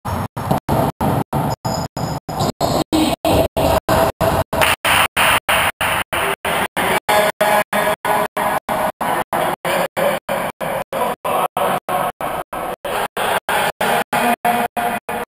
15 second clip Len's Trip 7-16-2014
Slow-mo clip of Len's backwards trip and bad shoulder hit.